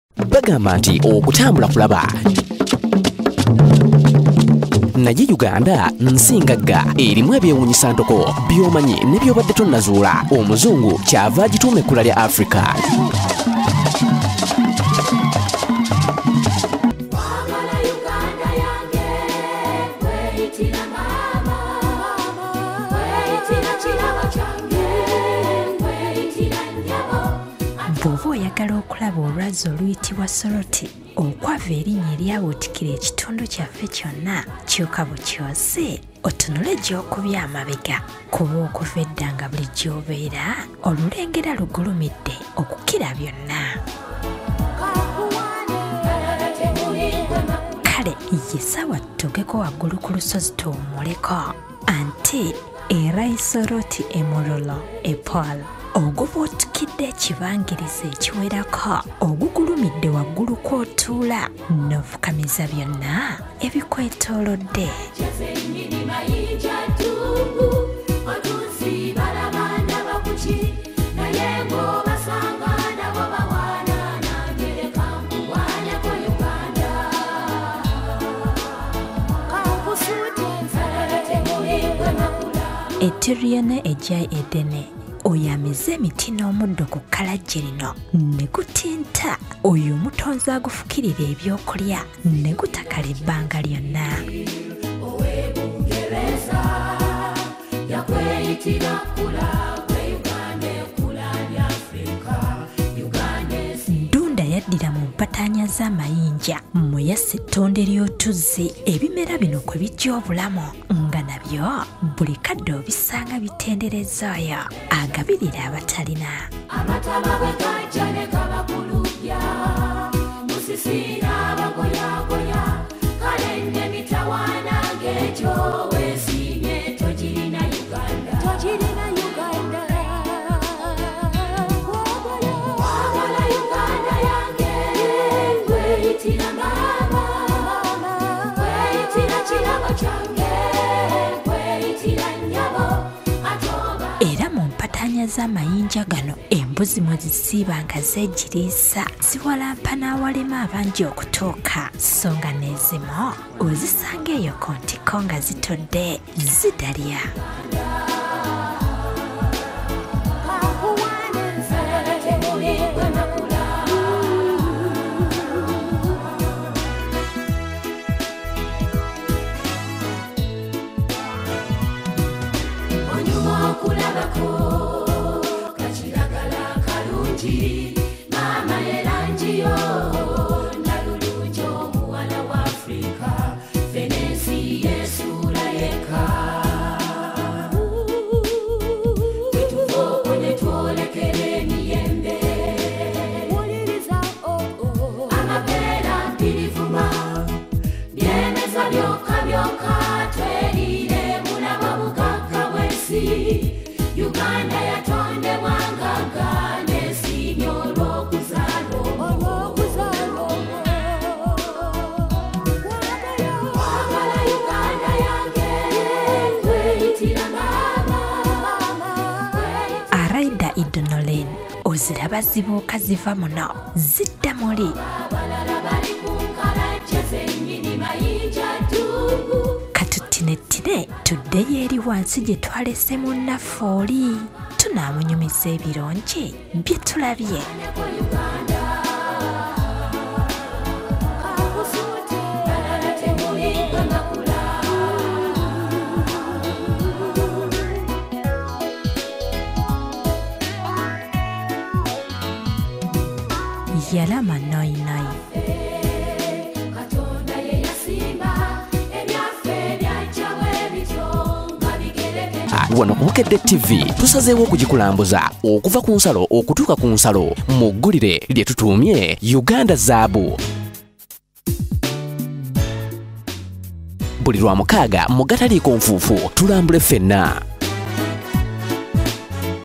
Uganada Zzaabu
Olwazi okwava erinnya ly’ekibuga Soroti